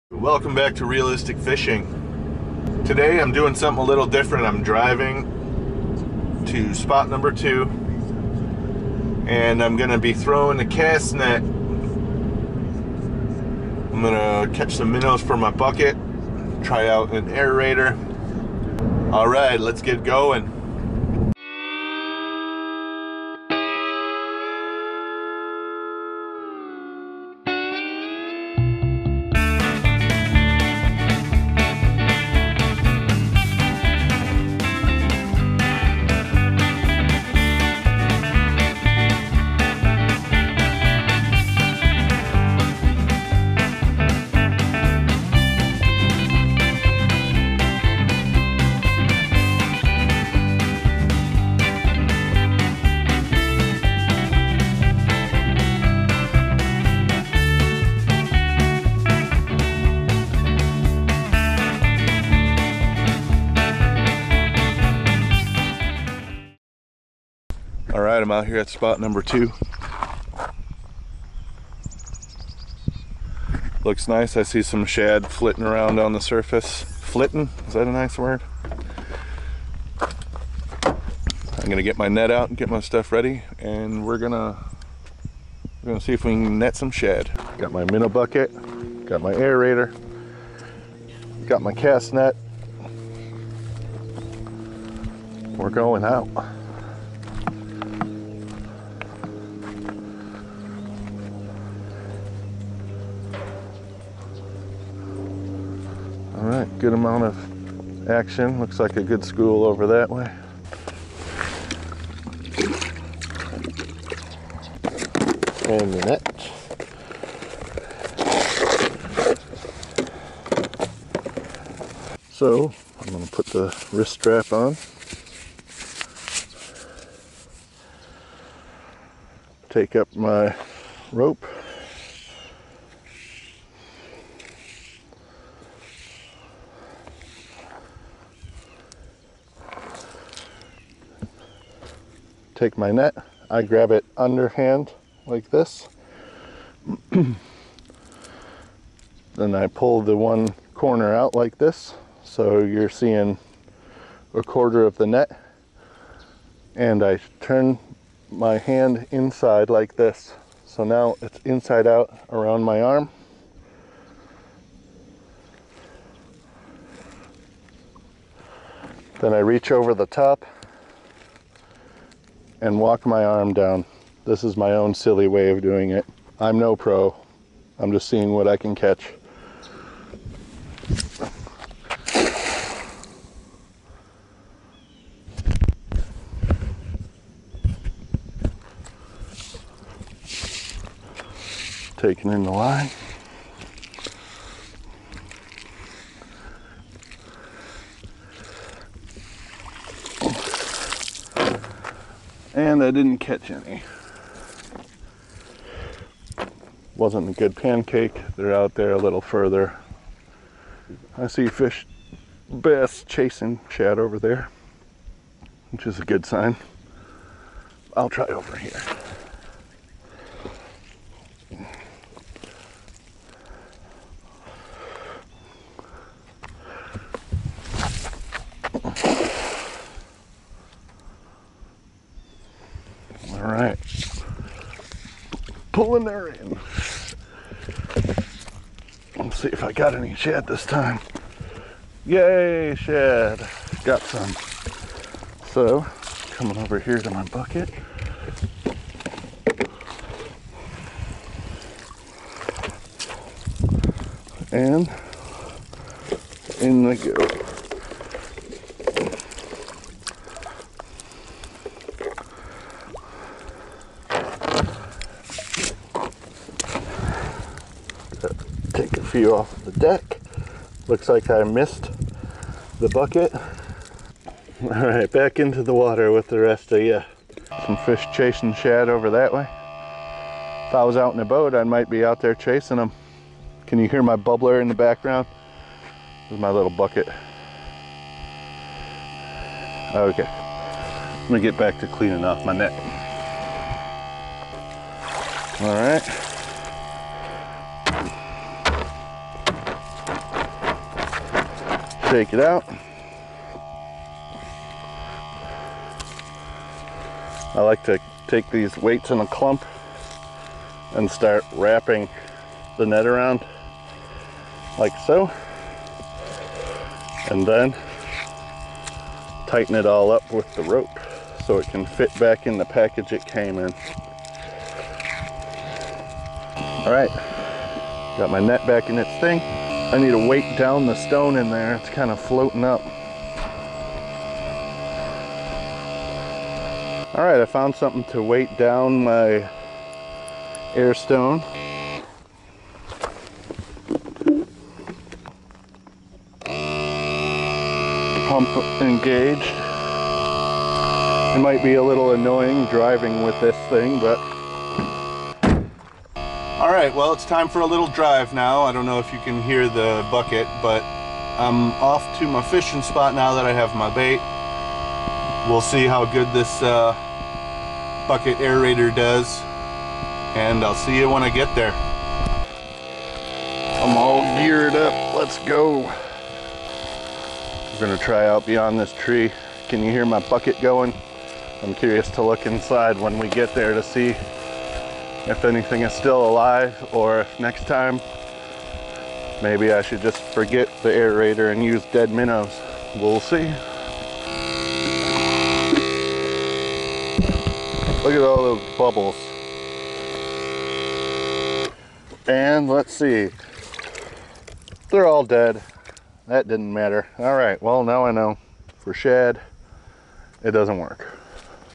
Catching Bait With A Cast Net - The Shad and Bucket Experiment
Can YouTube help me become a better fisherman? In this episode I am throwing a cast net in an attempt to catch shad. I manage to catch some shad and try to keep them alive in a bucket with an aerator pump. Can I make it to a different fishing spot with live shad that I caught in my cast net? Lets find out.